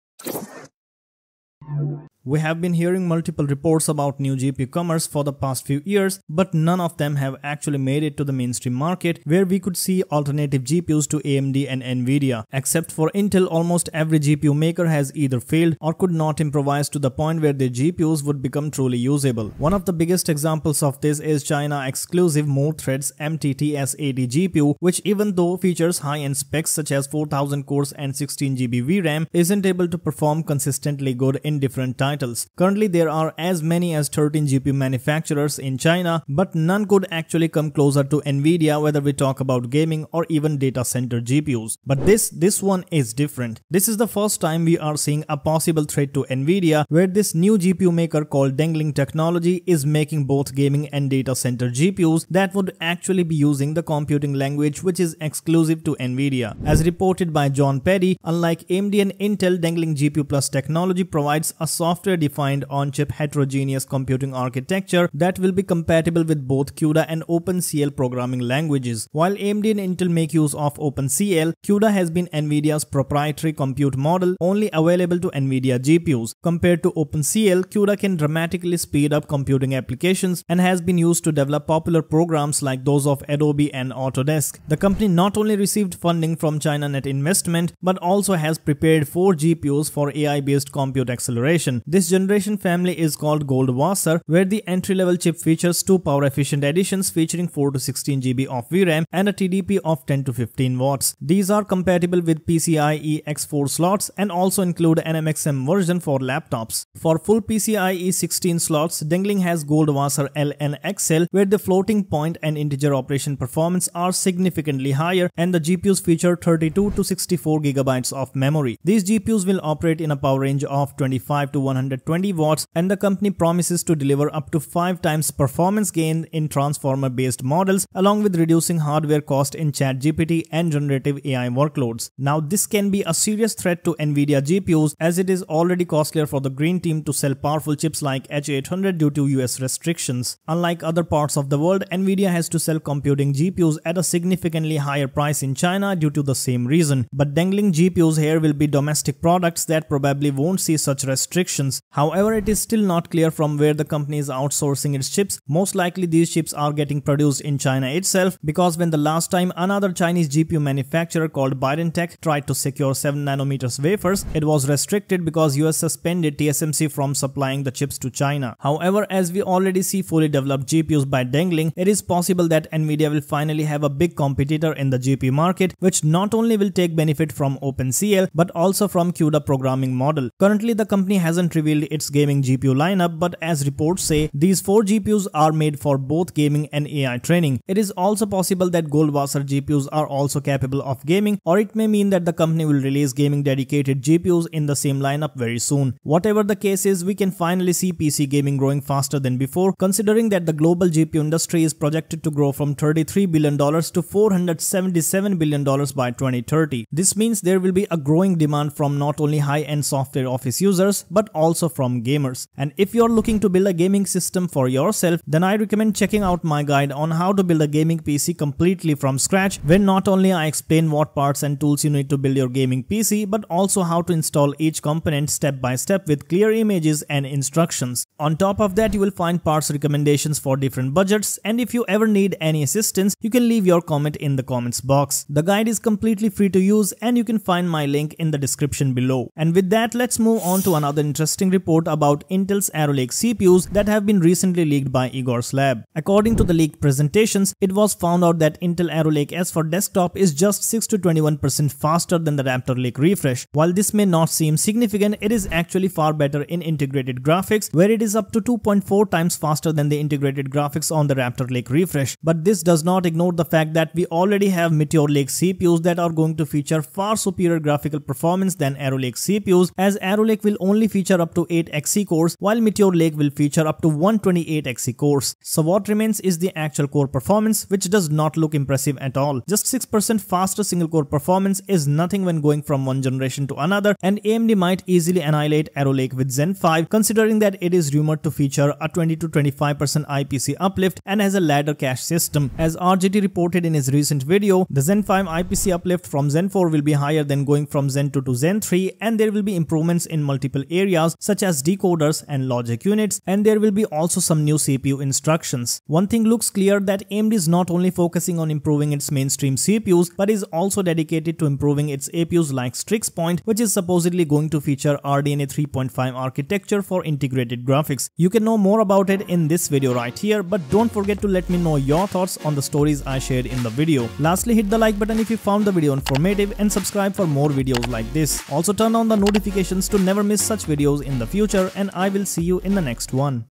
Nvidia Finally gets its Rival
A New GPU manufacturer has emerged in the market and is ready to take on Nvidia.